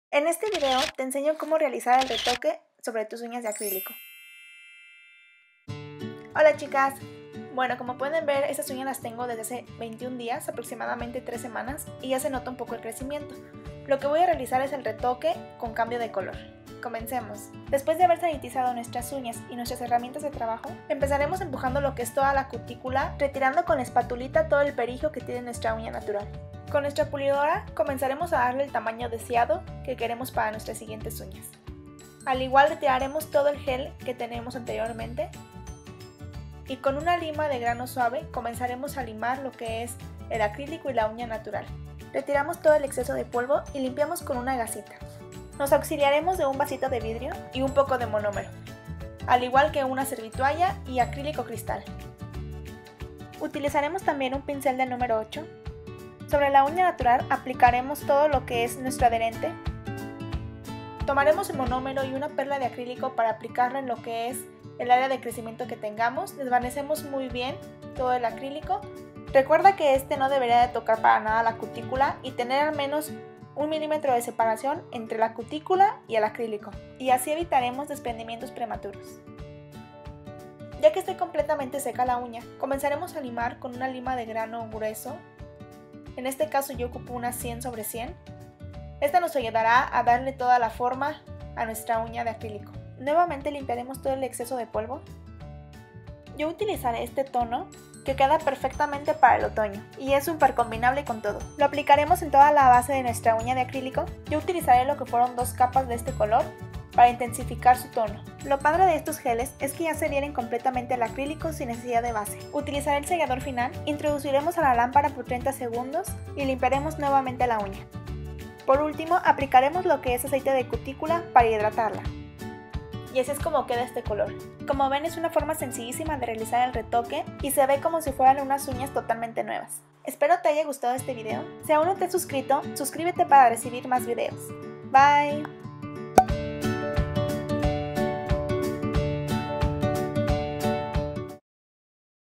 Retoque de Uñas | DIY
En este video te enseño como hacer un RETOQUE DE UÑAS con cambio de color de una forma facilisima y rapido, Si te sirvio este video no olvides SUSCRIBIRTE y darle LIKE